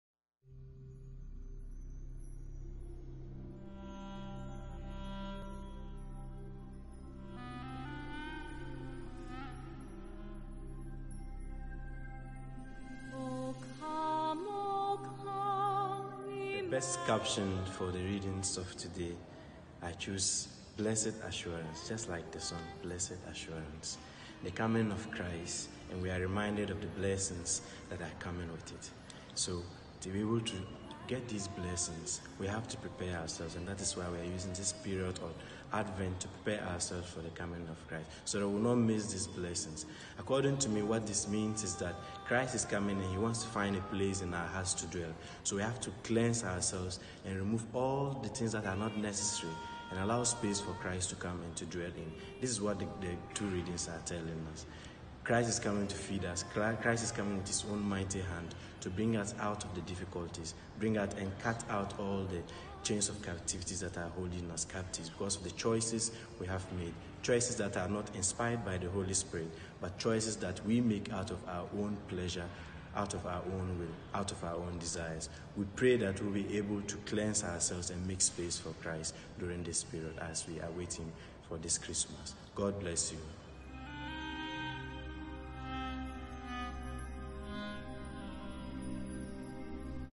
Advent, an assurance for Christmas
The compassion of God towards His people, prompts His Advent. He is coming in might to heal and feed us. What then are we waiting for? Let’s prepare our hearts for Him to come and heal our broken hearts and feed us with His word and His REAL presence in the Eucharist to replenish our strength for journey this season.